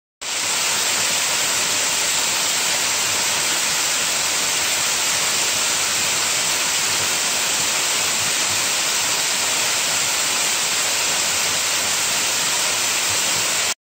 Margarette Falls Summer '20
The main view of the 60-foot waterfall in Cherokee National Forest.